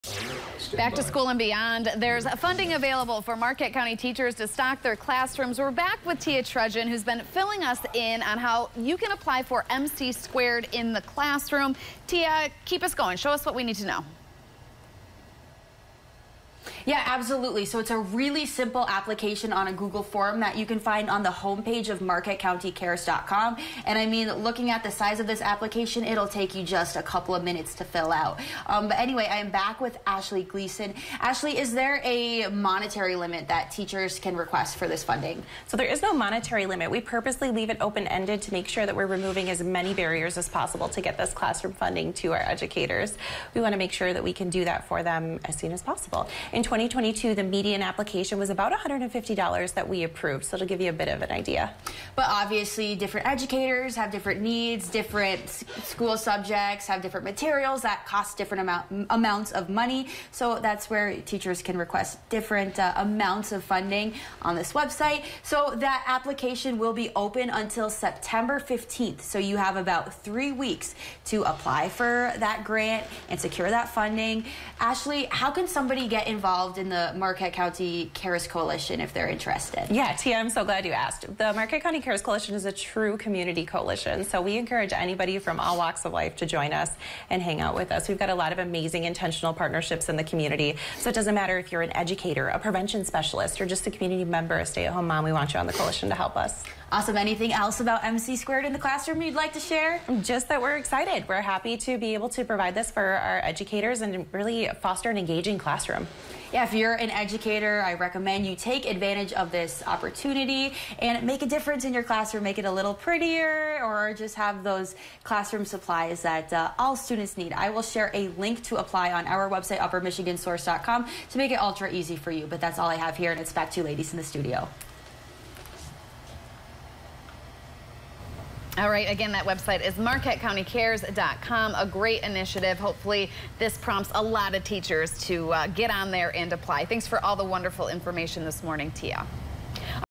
MC2 in the Classroom
TV6's Tia Trudgeon chats with the Marquette County Cares Coalition's Ashley Gleason about funds teachers can receive for their classrooms.